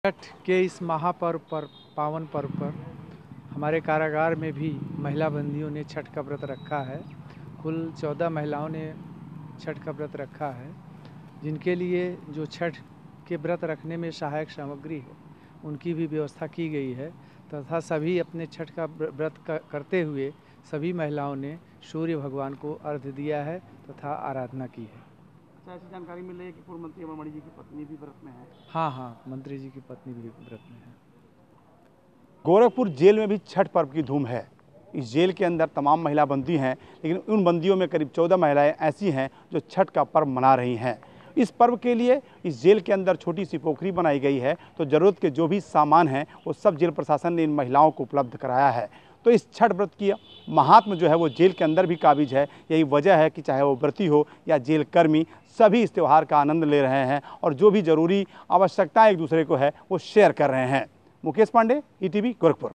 गोरखपुर: छठ महापर्व पर जेल में कैदी महिलाएं भी देंगी सूर्य को अर्घ्य
गोरखपुर: छठ महापर्व को मनाने के लिए कोई दीवार या बंधन अड़चन नहीं बन सकता. इसी की नजीर पेश की है गोरखपुर के मंडलीय कारागार में बंद बंदी महिलाओं ने. ये महिलाएं छठ व्रत को पूरे धूमधाम और विधि-विधान के साथ जेल परिसर के अंदर मना रही हैं. इन जेल बंदियों में कुल 14 महिलाएं छठ व्रत कर रही हैं.